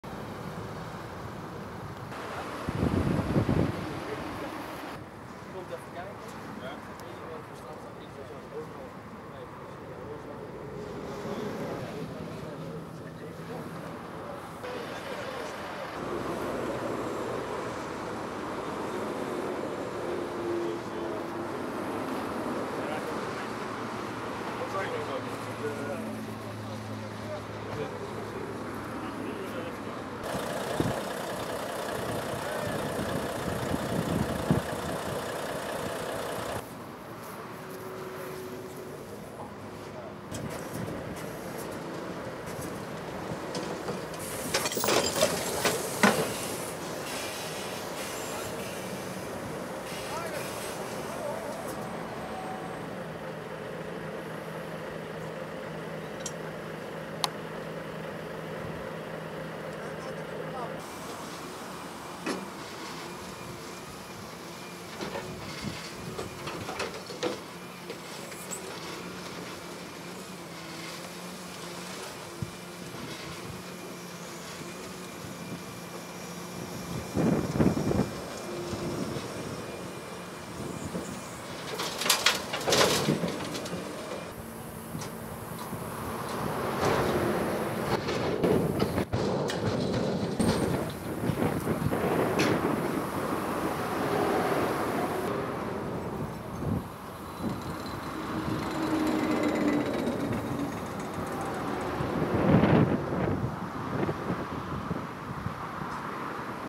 Ongeval vrachtwagen A73 Belfeld 18-09-2023
Belfeld - Een vrachtwagen is maandagmorgen deels gekanteld bij het nemen van de toerit van de A73 bij Belfeld richting Venlo. Volgens een correspondent ter plaatse is de chauffeur ongedeerd gebleven, hij is ter plaatse gecontroleerd door een verpleegkundige van de ambulancedienst.

De vrachtwagen vervoerde afval, groot deel daarvan is in de berm terechtgekomen. Ook is er olie op de weg beland. Volgens Rijkswaterstaat gaat de toerit pas rond middernacht weer open, omdat het asfalt moet worden vernieuwd.